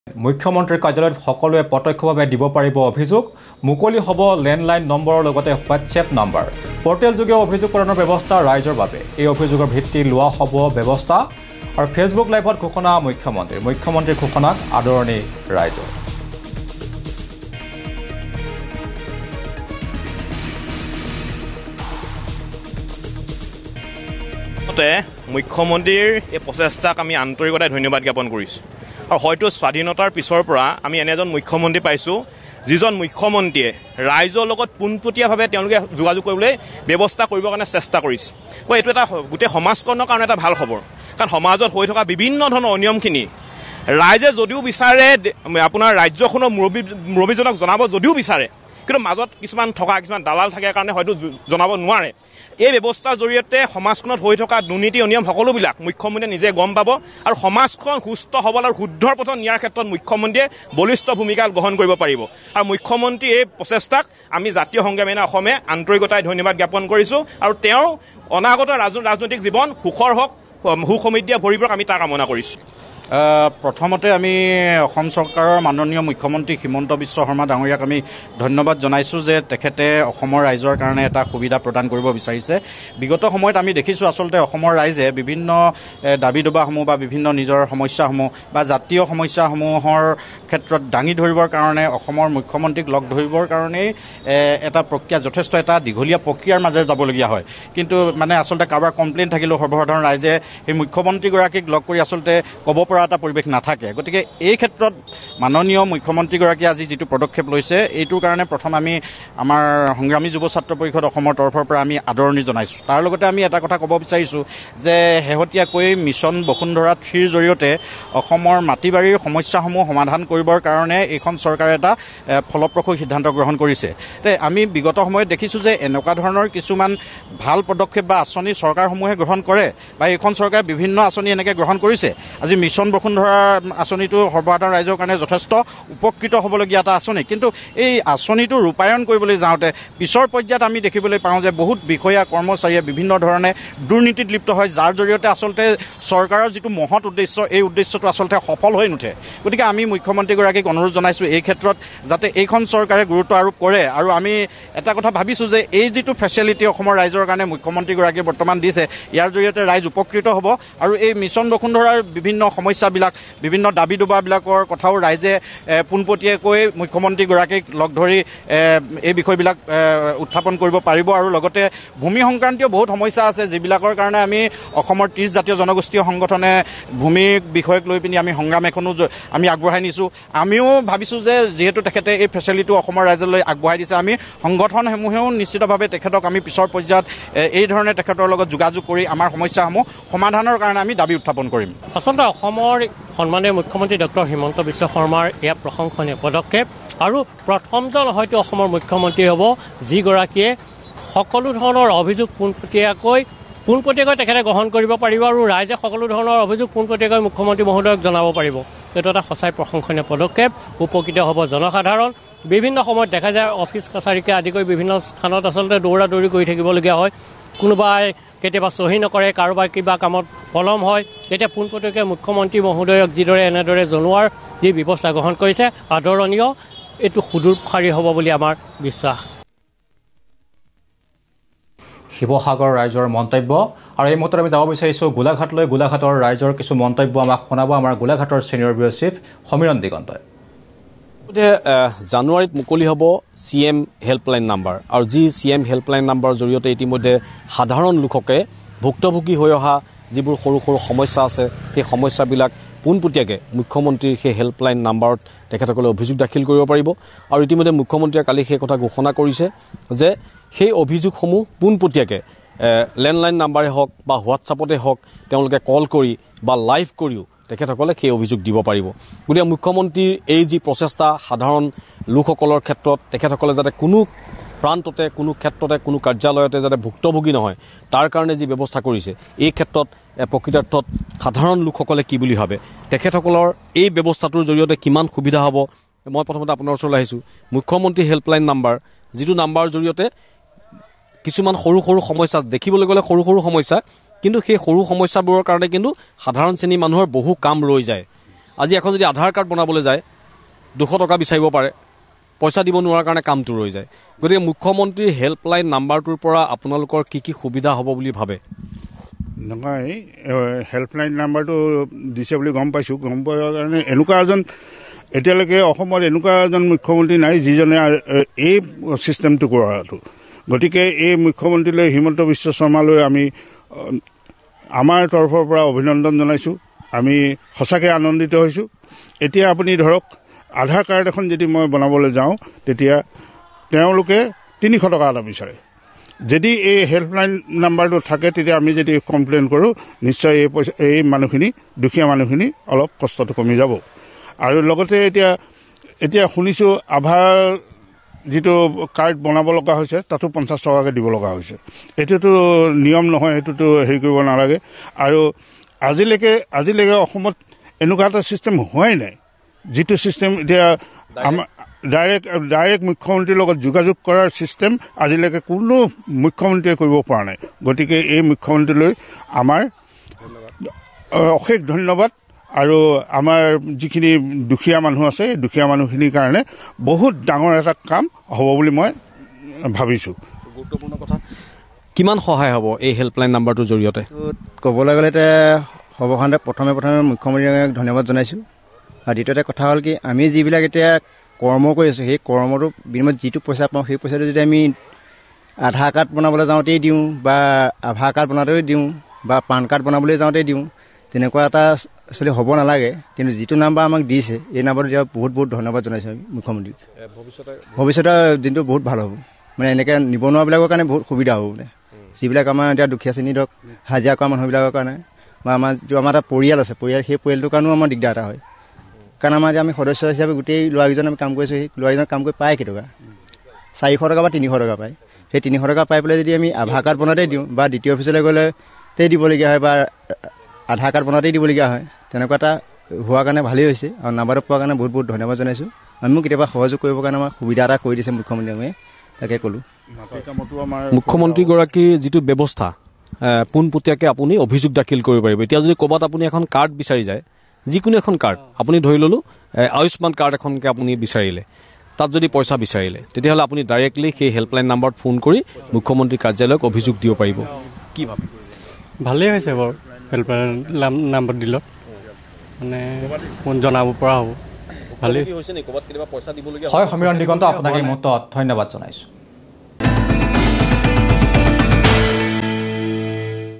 মুখ্যমন্ত্ৰীৰ ঘোষণাক আদৰণি ৰাইজৰ
📌মুখ্যমন্ত্ৰীৰ কাৰ্যালয়ত সকলোৱ প্ৰত্যক্ষভাৱে দিব পাৰিব অভিযোগ